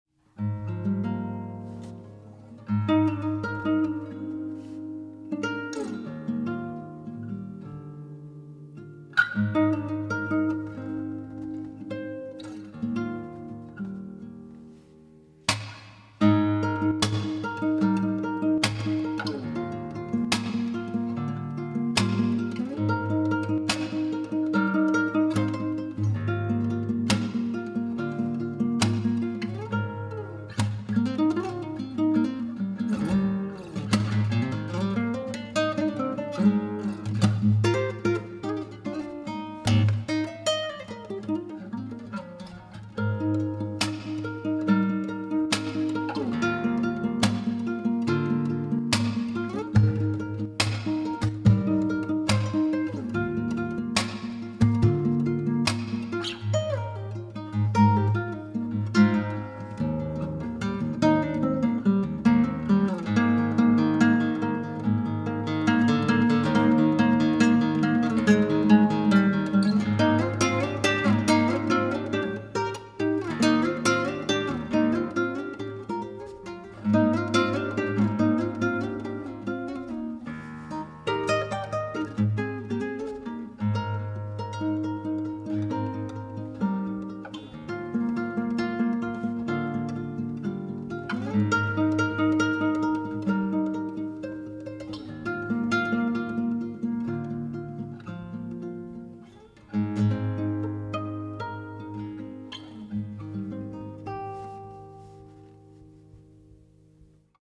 ALTAMIRA BASICO 7/8 Nat klasična gitara
ALTAMIRA BASICO 7/8 klasična gitara je početni model u obitelji Altamira gitara. Unutarnja konstrukcija Basica odgovara konstrukciji N300, a izvana se razlikuju po tome što Basico ima mat površinu. Prednja ploča od punog cedra daje topao i prodoran zvuk koji nećete pronaći u ovom cjenovnom razredu.

ALTAMIRA BASICO je gitara prikladna za početnike koji počinju učiti klasičnu gitaru. Basico 7/8 s mat završnom obradom odlikuje se toplim i prodornim tonom, jednostavnom estetikom, te kvalitetnom prednjom pločom od punog cedra. Prednja ploča gitare važna je komponenta u stvaranju zvuka, a kod gitara s punom prednjom pločom, što zvučna ploča više vibrira, to se više ovaj pojedinačni komad drveta prilagođava i "otvara", što poboljšava kvalitetu zvuka kako gitara stari.